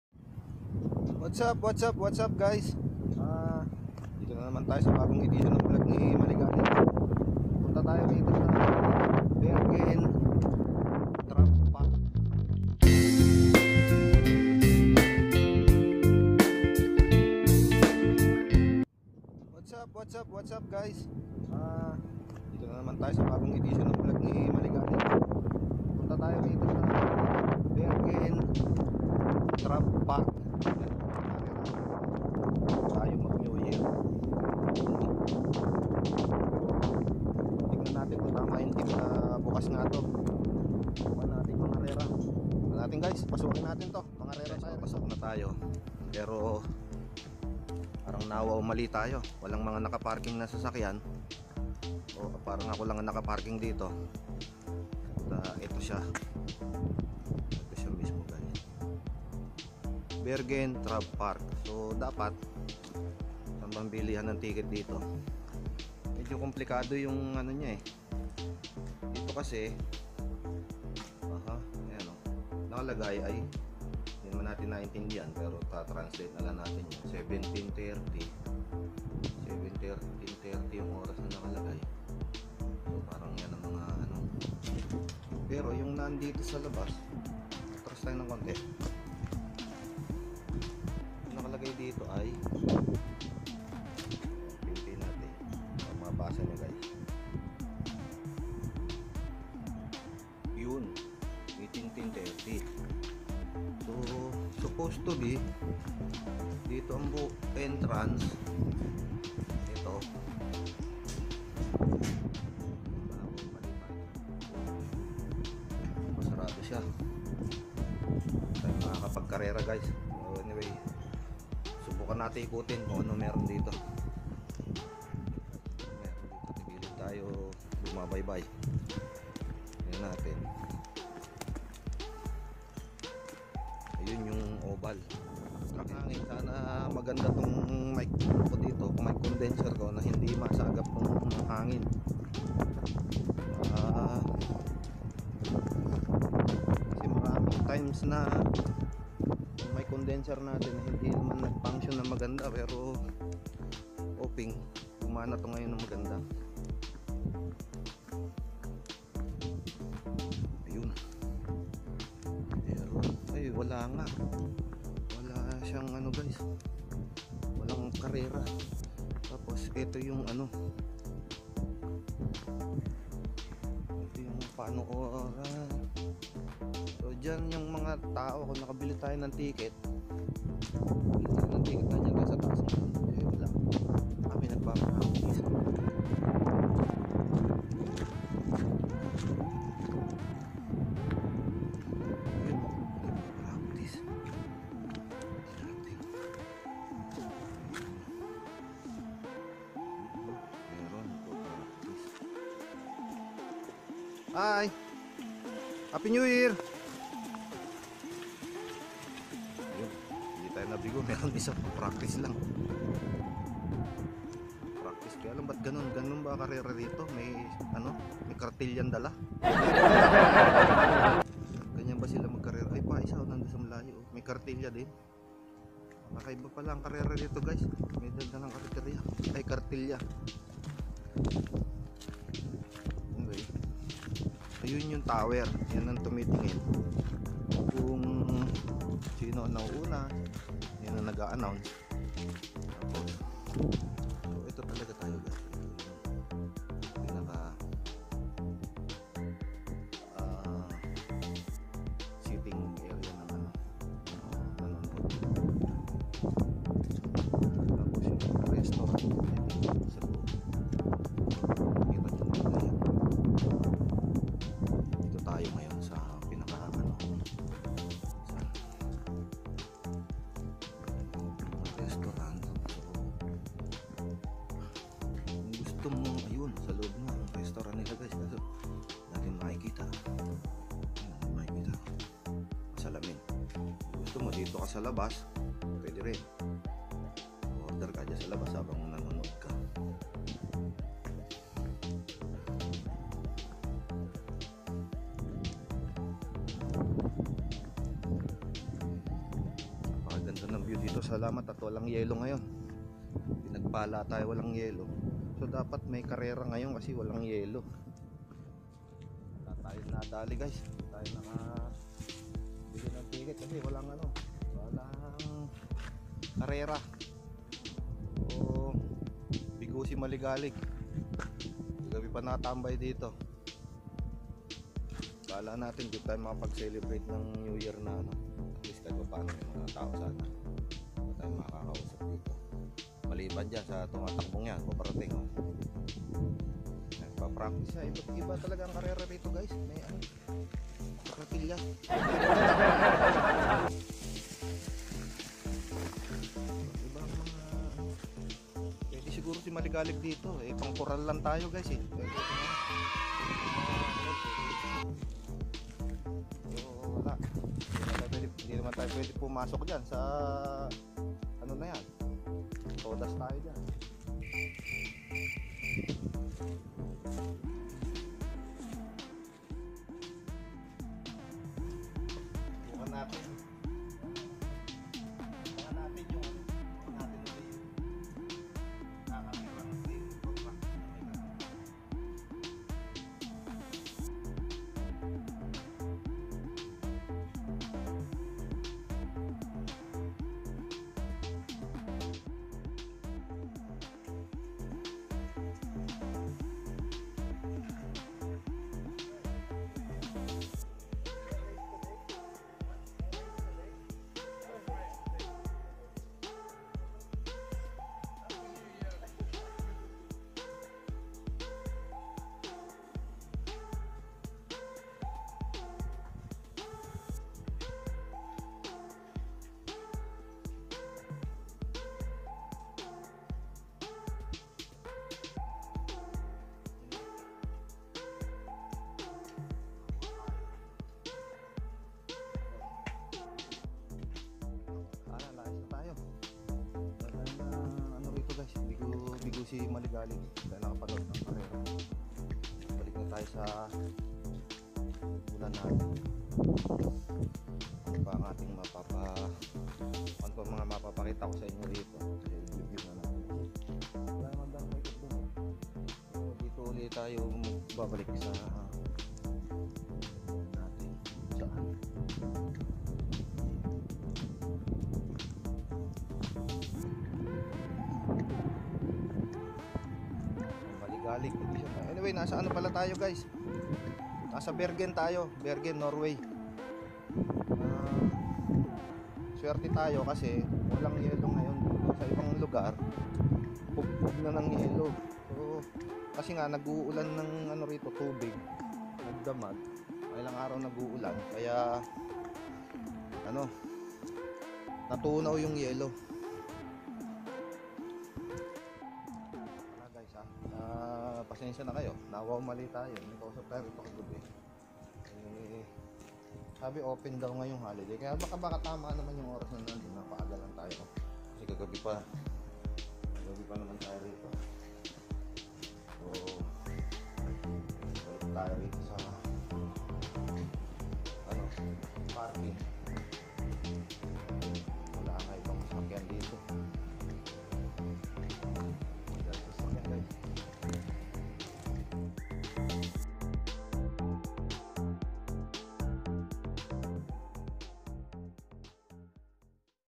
karerahan ng kabayo sa Norway na wow mali si maligalig
Edit

Bergen Travpark is a harness racing track located at Åsane in Bergen, Norway. The course is 1,000 meters (3,300 ft). Owned by Norwegian Trotting Association, its tote betting is handled by Norsk Rikstoto. The venue opened on 15 June 1985.[1]
Bergen TravparkLocationÅsane, Bergen, NorwayOwned byNorwegian Trotting AssociationDate opened15 June 1985Course typeHarness racing